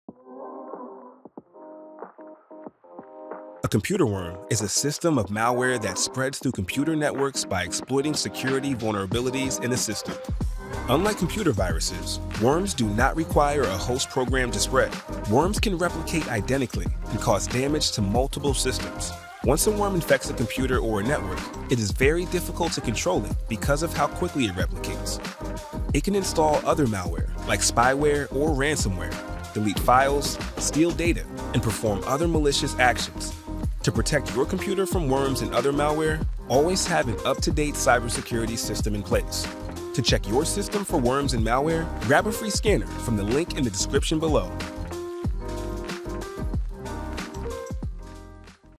What is a Computer Worm
A computer worm is a type of malware that spreads through computer networks by exploiting security vulnerabilities in the system. Unlike computer viruses, worms do not require a host program to spread. Worms can replicate independently and cause damage to multiple systems.

Watch this video to learn more about computer worms, how they can harm your systems and how to protect your computer.

Video breakdown:
0:00 Intro
0:03 What is a computer worm
0:11 How a computer worm is different from a virus
0:20 How a worm can harm your computer
0:34 How to protect your computer